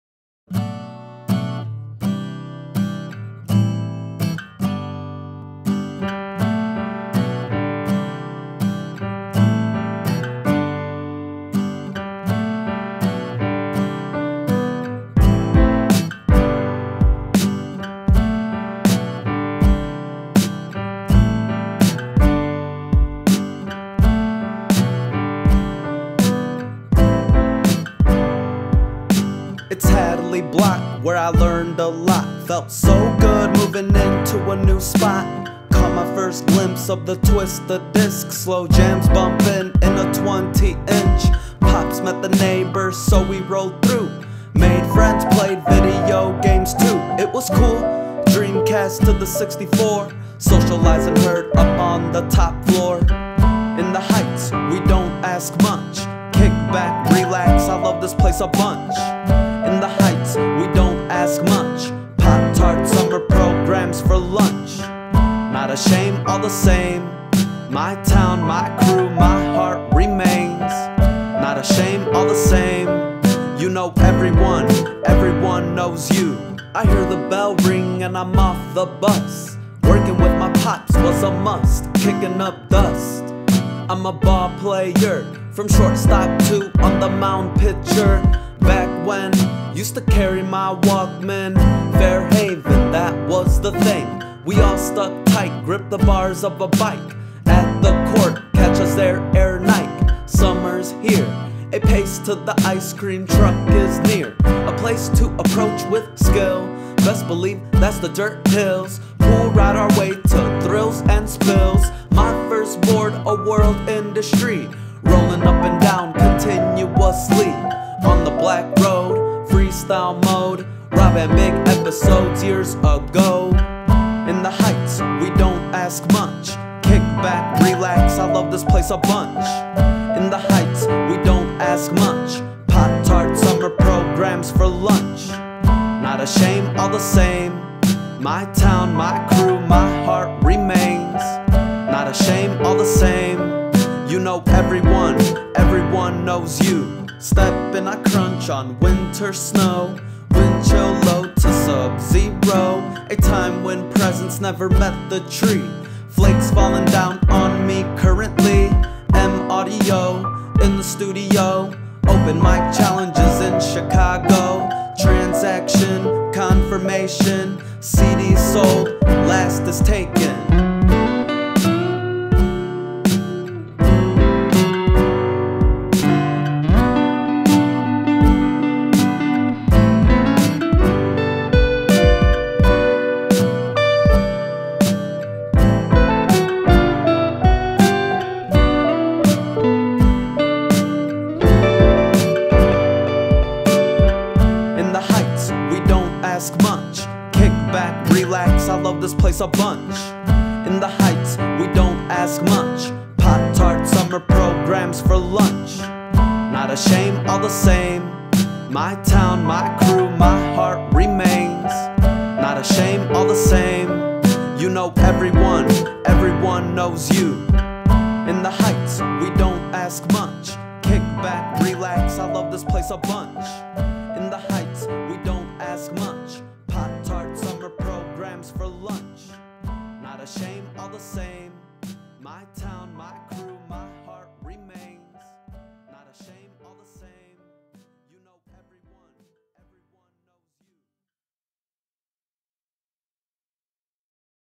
The Heights by agpnodoubt
Its where my heart is. My small town Kingsford Heights, IN.